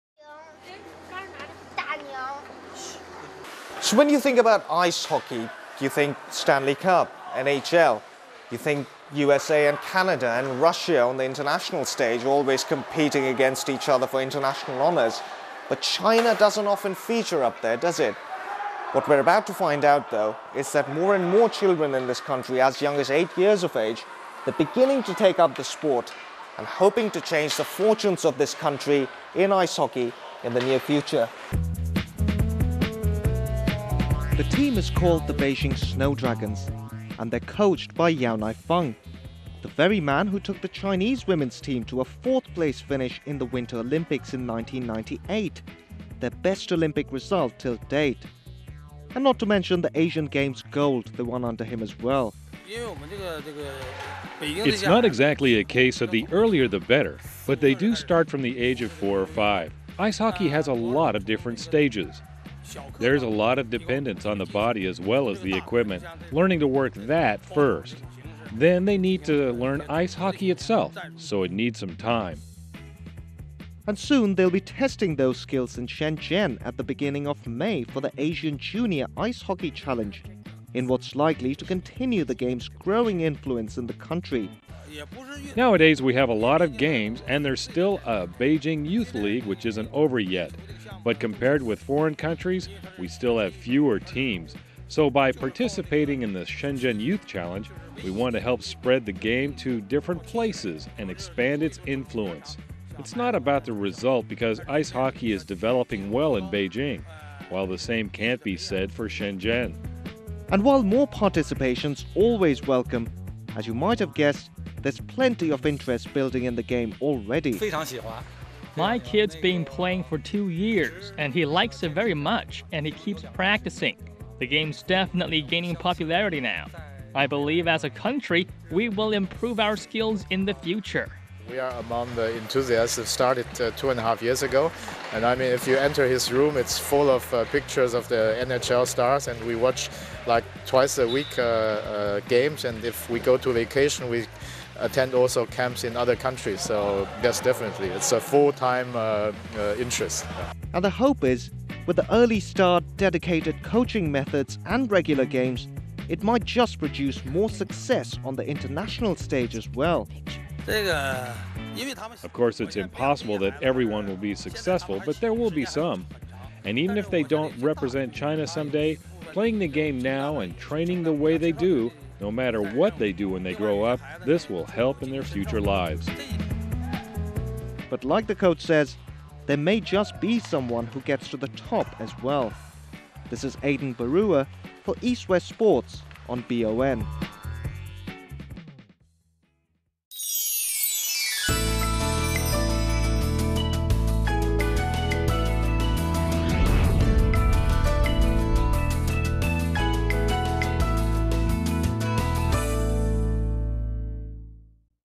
Beijing Junior Ice Hockey - Blue Ocean Network (BON)
A look at a youth ice-hockey side from Beijing coached by former Chinese Women's team coach, Yao Naifeng, ahead of their participation in the Asian Youth Challenge in Shenzhen